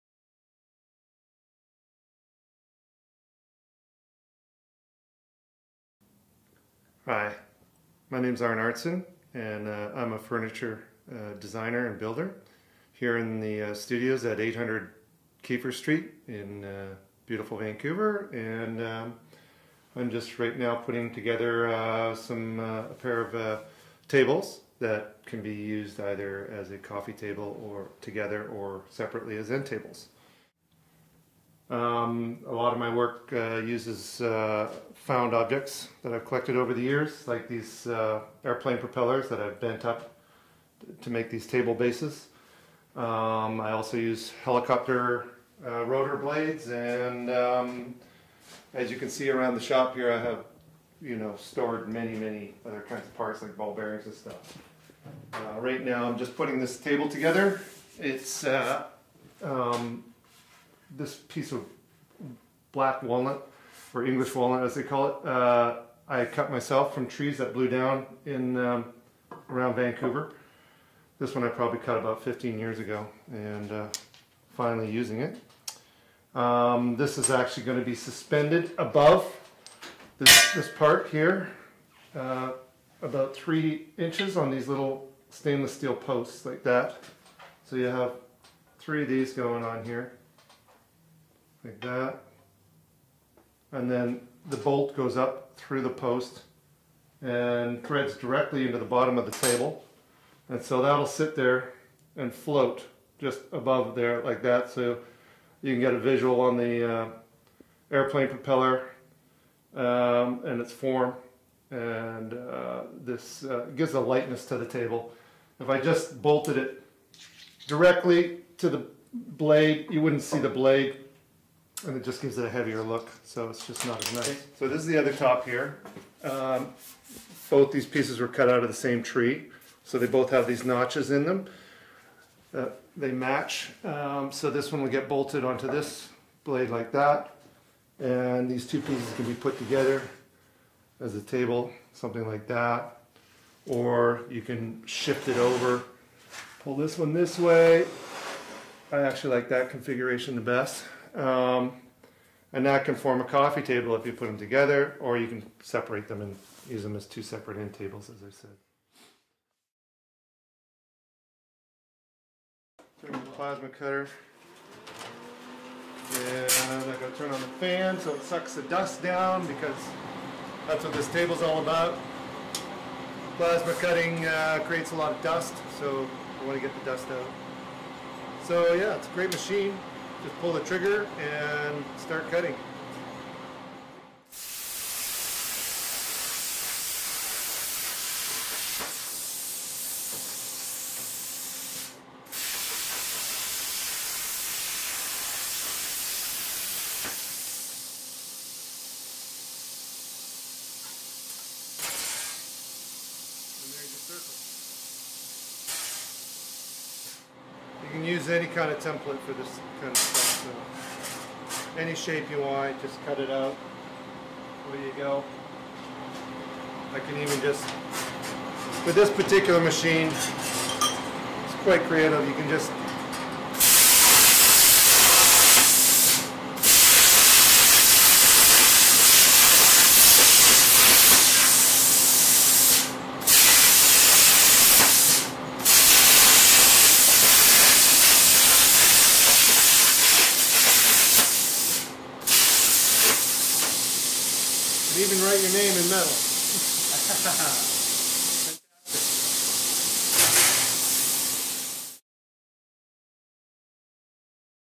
ArntArntzenInterview800Keefer14Nov2011
Short Interview with metal sculptor and furniture designer, Arnt Arntzen. Shot at AMP Studio, 800 Keefer, St, Vancouver, BC, Canada. He discusses: inspiration; use of found materials and aircraft parts; and explains a work in progress. Filmed & Edited by Jeff Murton in the week prior to the 2011 East Side Culture Crawl.